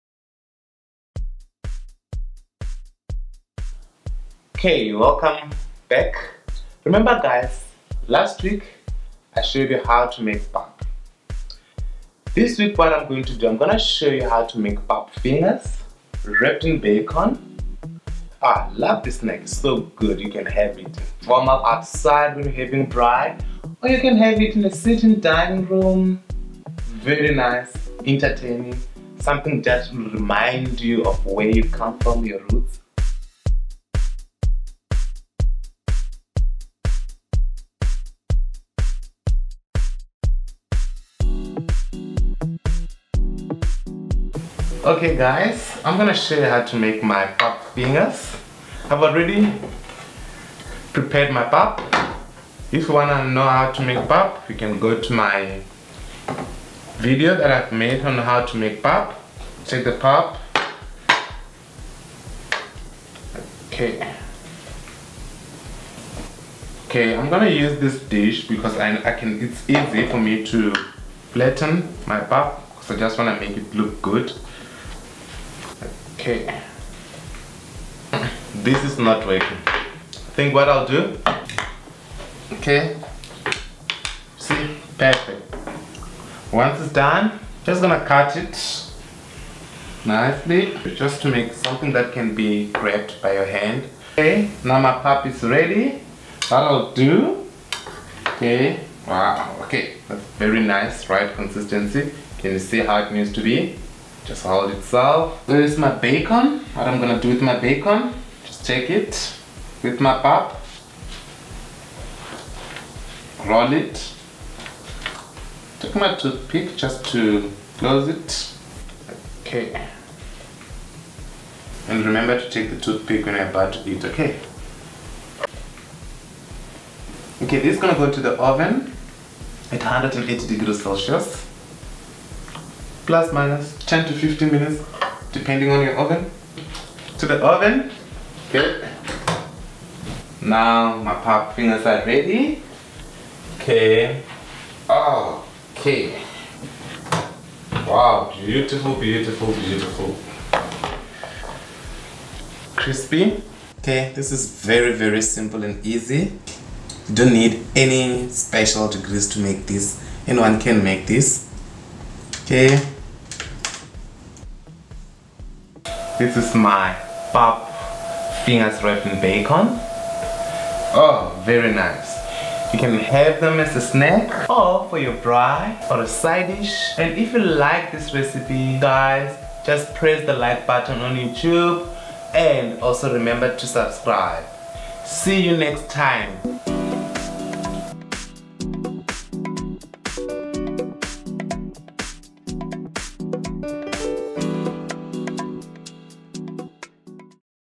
Pap Fingers
Watch Funi make these delicious pap fingers wrapped in bacon. Yummy. A great braai snack.